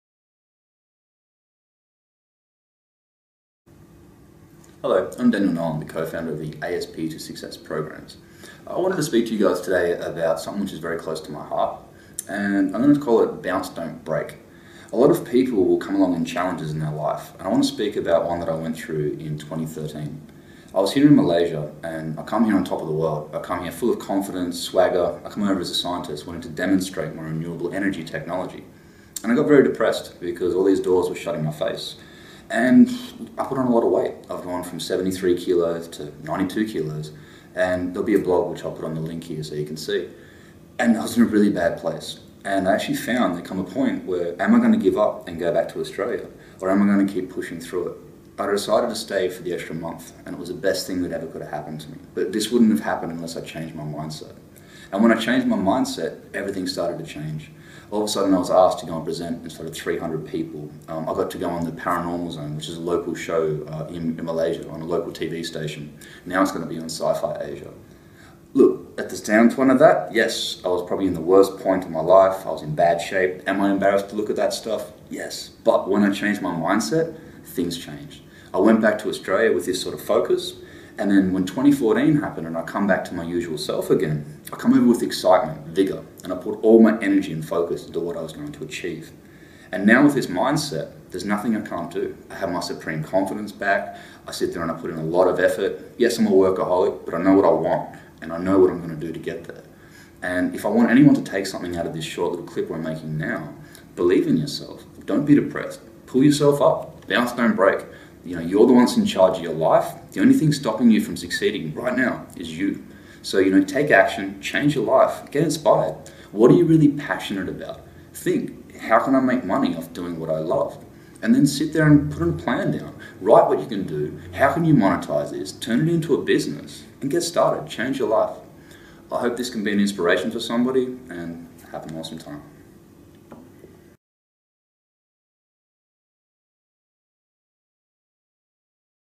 Bounce Dont Break and Take Action
A friend wanted me to chat about how I face my challenges with a bounce don't break attitude when life gets rough. Things in life are always going to get up down, but it is how we face these challenges and rise above it that will define us.
We have a choice to either be negative and hold on to a bad situation or to change our mindset to a positive one.
The only limitations are the ones we place on ourselves!

Hard work, dream big, make it count!